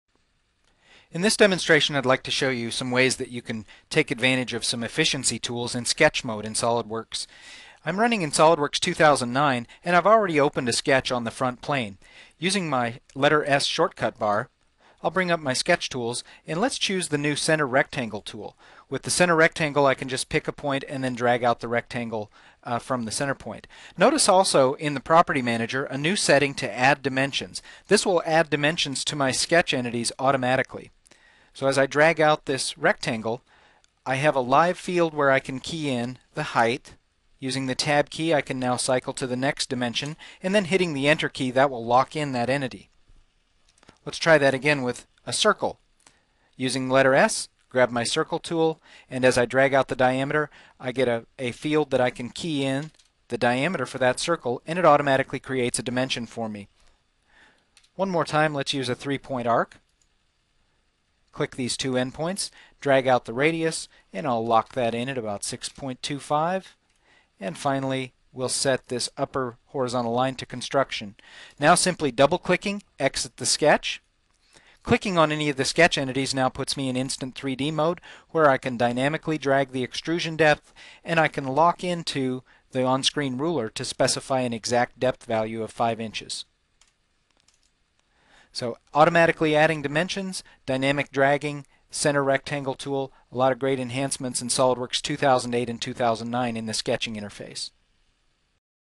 Add Sketch Dimensions Automatically
How to use the new Add Dimensions setting to automatically add sketch dimensions to any sketch entity in SolidWorks 2009.